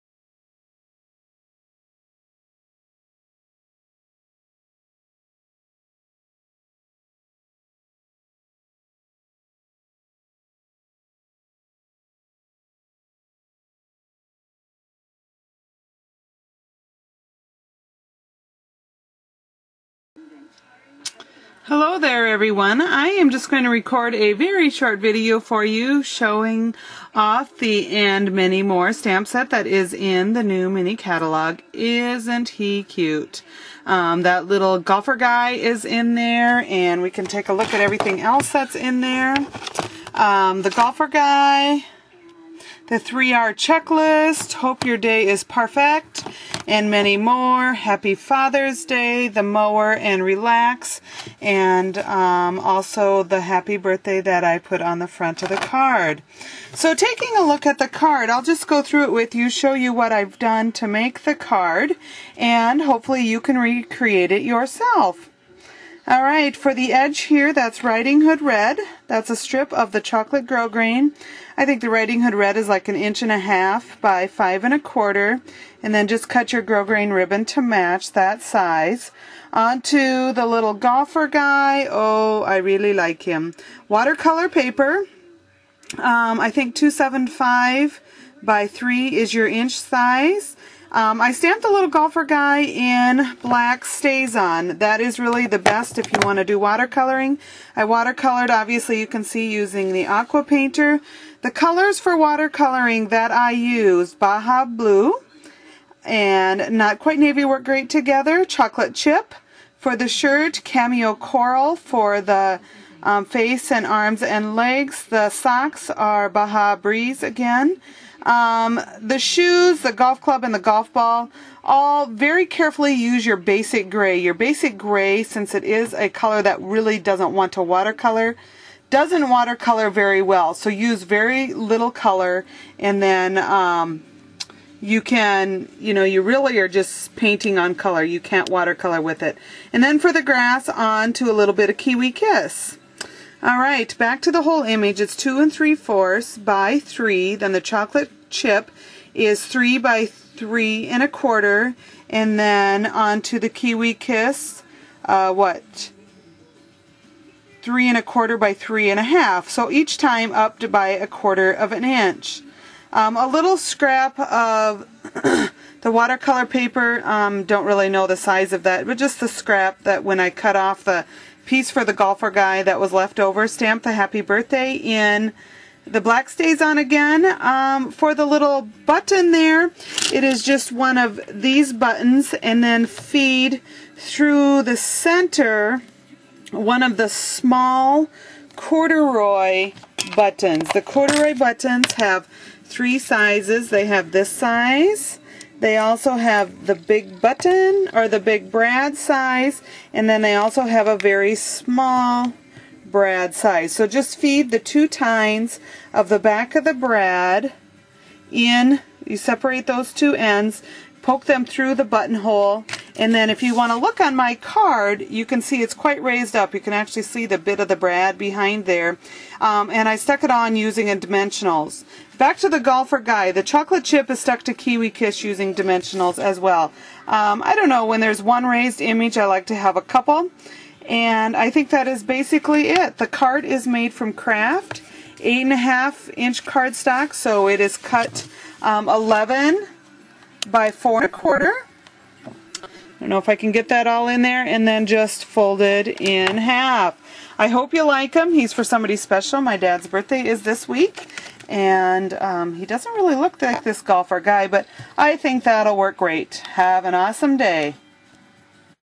The Golfer Guy
A card made using the And Many Mower stamp set from Stampin' Up!  You can view more of my projects